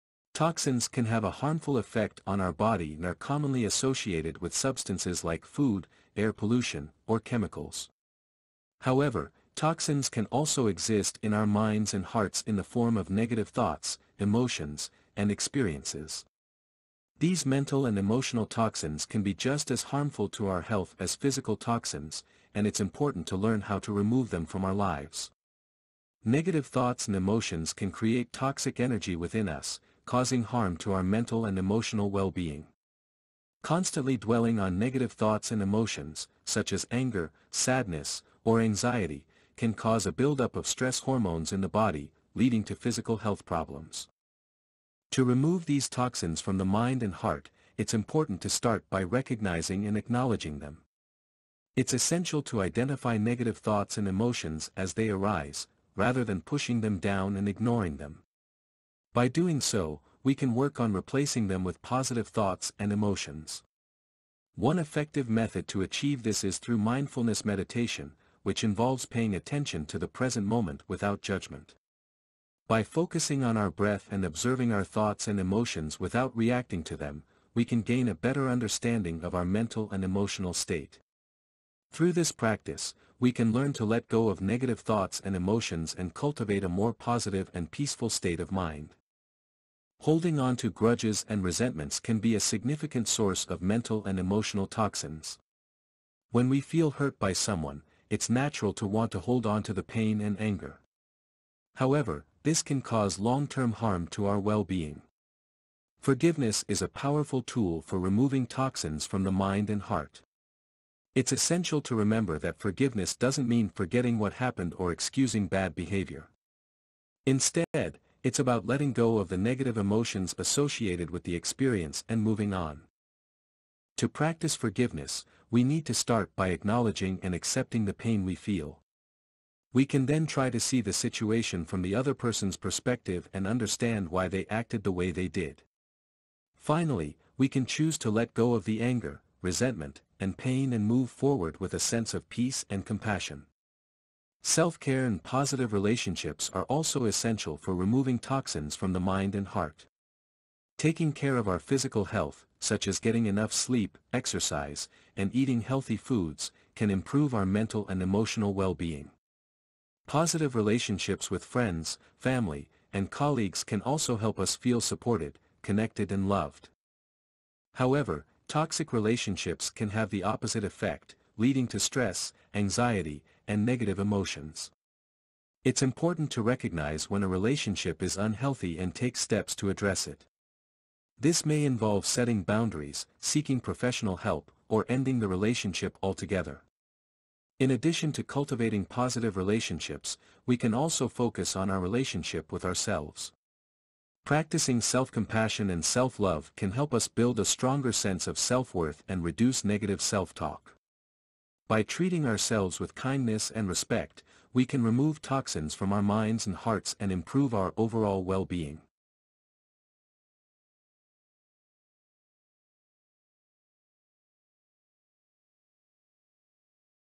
How to Remove Toxins from Your Mind and Heart
Toxins are substances that can cause harm to our body, and we often think of them in terms of food, air pollution, or chemicals. However, toxins can also exist in our minds and hearts in the form of negative thoughts, emotions, and experiences. These mental and emotional toxins can be just as harmful to our health as physical toxins, and it's important to learn how to remove them from our lives.

Negative thoughts and emotions can create toxic energy within us, causing harm to our mental and emotional wellbeing. Constantly dwelling on negative thoughts and emotions, such as anger, sadness, or anxiety, can cause a build-up of stress hormones in the body, leading to physical health problems. To remove these toxins from the mind and heart, we need to start by recognizing and acknowledging them. It's important to identify negative thoughts and emotions as they arise, rather than pushing them down and ignoring them. We can then work on replacing them with positive thoughts and emotions.

One way to do this is through mindfulness meditation, which involves paying attention to the present moment without judgment. By focusing on our breath and observing our thoughts and emotions without reacting to them, we can gain a better understanding of our mental and emotional state. Through this practice, we can learn to let go of negative thoughts and emotions and cultivate a more positive and peaceful state of mind.

Holding onto grudges and resentments can be a significant source of mental and emotional toxins. When we feel hurt by someone, it's natural to want to hold onto the pain and anger. However, this can cause long-term harm to our wellbeing. Forgiveness is a powerful tool for removing toxins from the mind and heart. It's important to remember that forgiveness doesn't mean forgetting what happened or excusing bad behavior. Instead, it's about letting go of the negative emotions associated with the experience and moving on.

To practice forgiveness, we need to start by acknowledging and accepting the pain we feel. We can then try to see the situation from the other person's perspective and understand why they acted the way they did. Finally, we can choose to let go of the anger, resentment, and pain and move forward with a sense of peace and compassion.

Self-care and positive relationships are also essential for removing toxins from the mind and heart. Taking care of our physical health, such as getting enough sleep, exercise, and eating healthy foods, can improve our mental and emotional wellbeing. Positive relationships with friends, family, and colleagues can also help us feel supported, connected, and loved. However, toxic relationships can have the opposite effect, leading to stress, anxiety, and negative emotions. It's important to recognize when a relationship is unhealthy and take steps to address it. This may involve setting boundaries, seeking professional help, or ending the relationship altogether.

In addition to cultivating positive relationships, we can also focus on our relationship with ourselves. Practicing self-compassion and self-love can help us build a stronger sense of self-worth and reduce negative self-talk. By treating ourselves with kindness and respect, we can remove toxins from our minds and hearts and improve our overall wellbeing.

Removing toxins from the mind and heart is essential for maintaining good mental and emotional health. By identifying negative thoughts and emotions, practicing forgiveness, cultivating positive relationships, and practicing self-care, we can reduce the harmful effects of mental and emotional toxins and improve our overall wellbeing. It's important to remember that removing toxins from the mind and heart is an ongoing process. It's not something that can be accomplished overnight, but rather a lifelong journey.